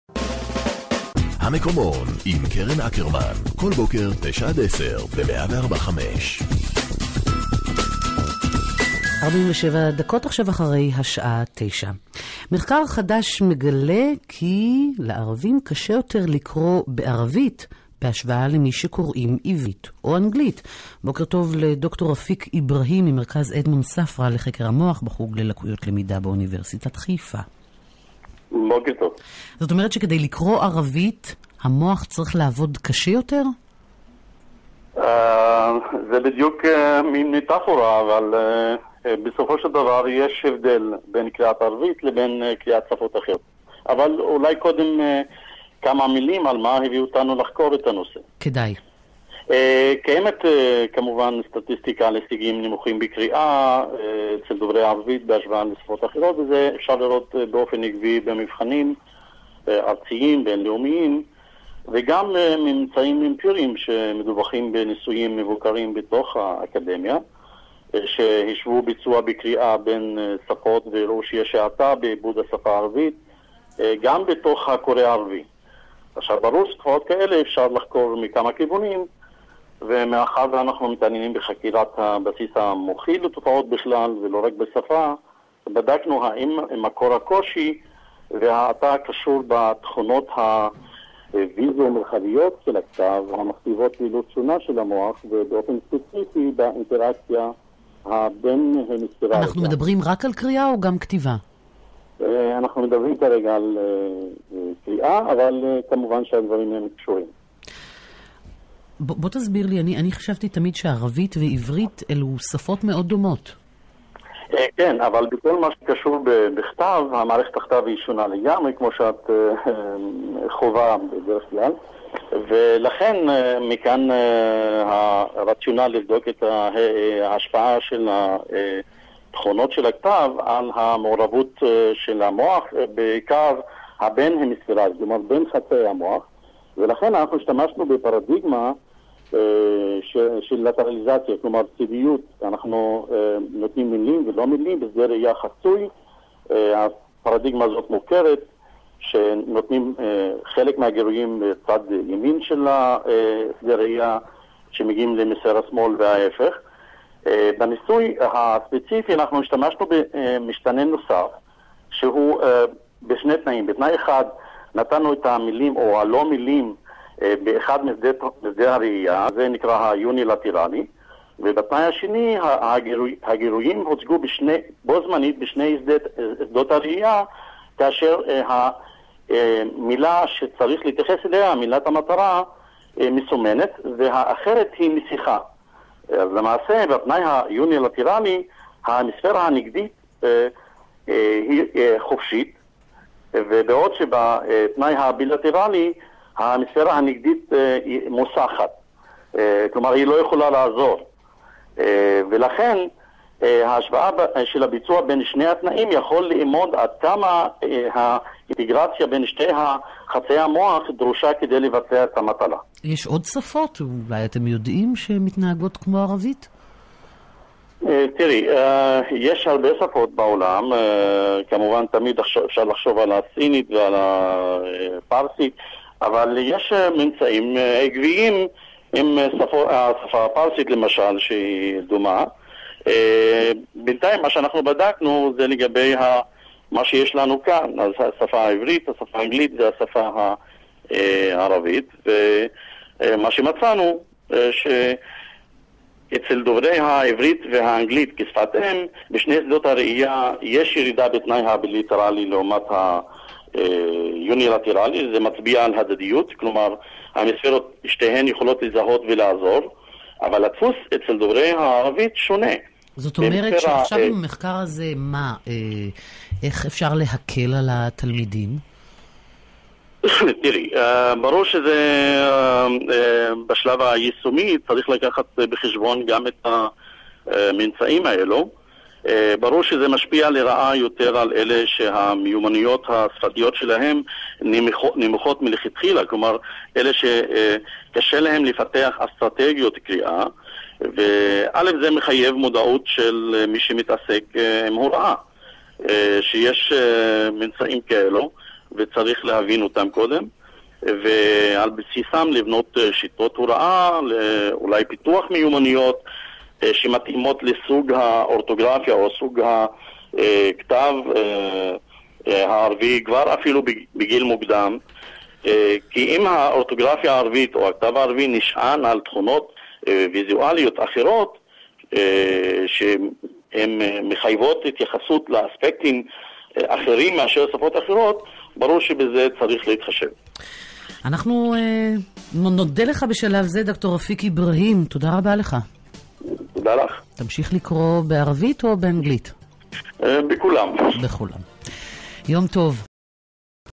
الدكتور رفيق ابراهيم يكشف
الدكتور رفيق ابراهيم يكشف في دراسة جديدة توزيع الوظائف بين النصف الايمن و النصف الايسر من الدماغ في معالجة اللغة المكتوبة .